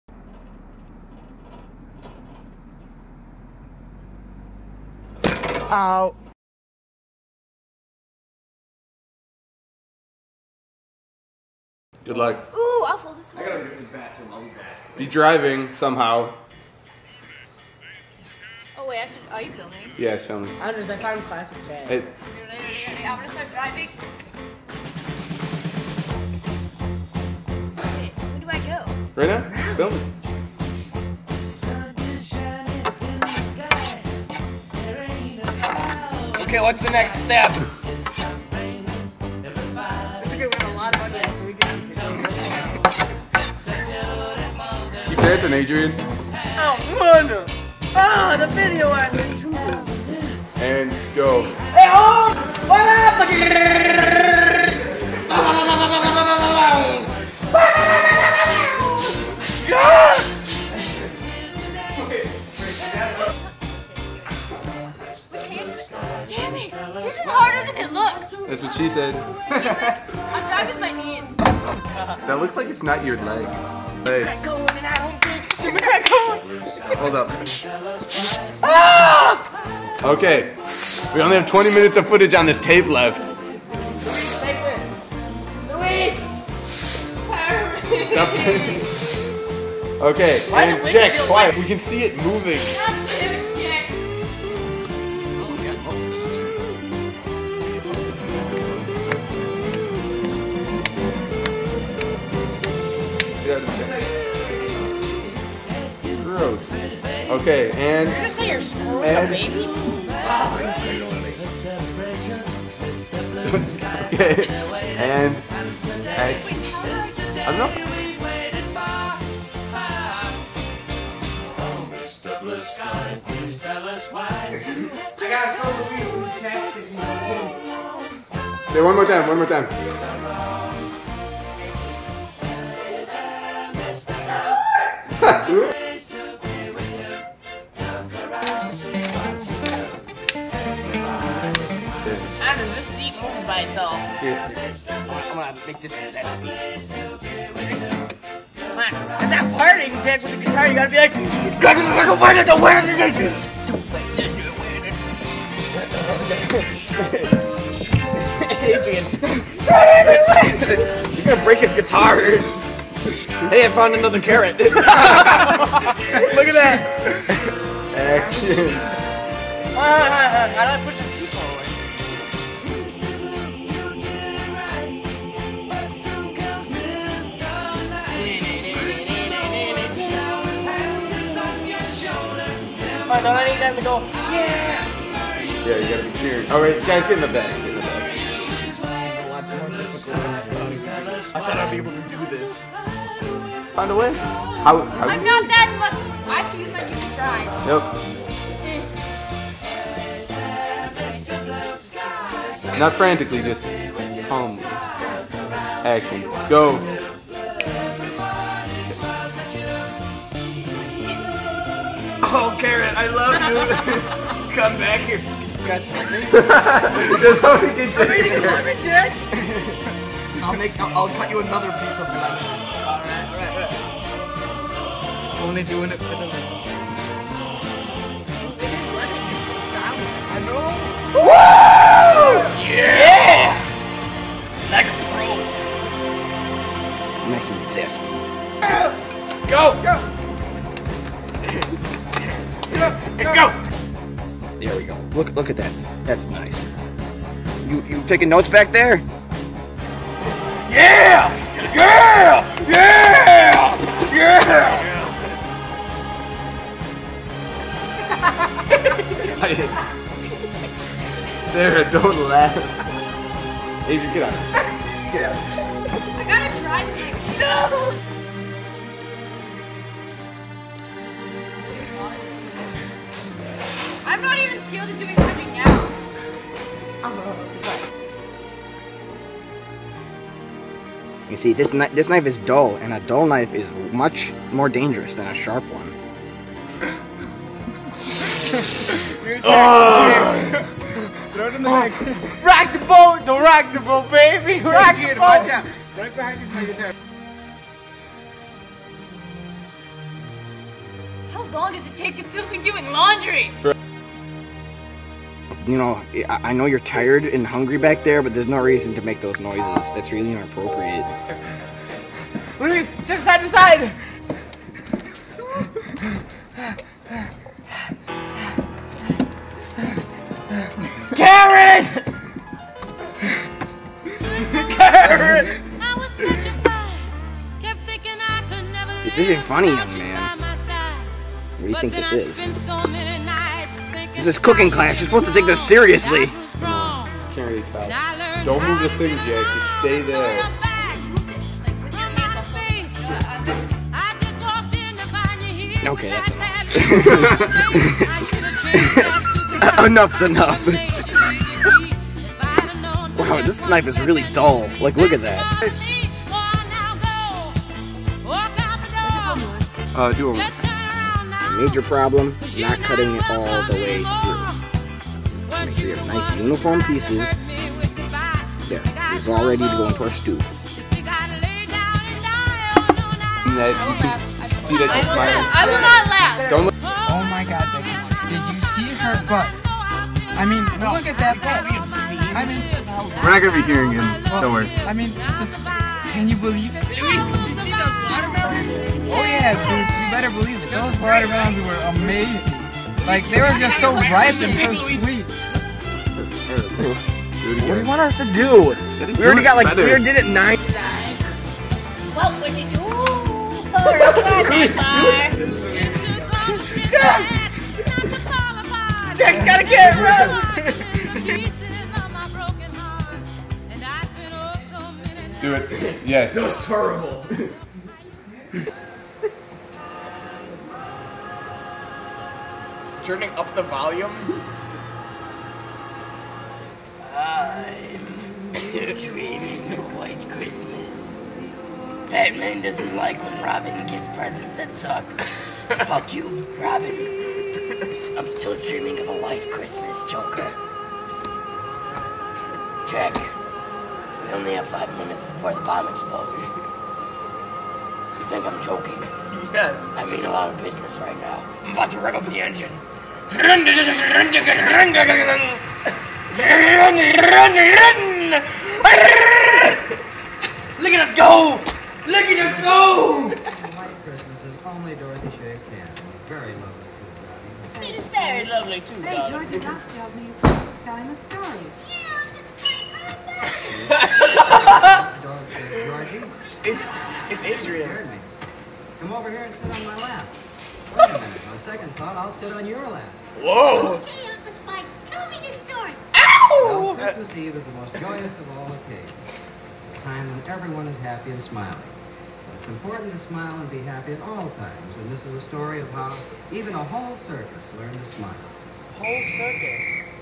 Xtreme Distracted Driving Outtakes
Outtakes from our video warning about distracted driving.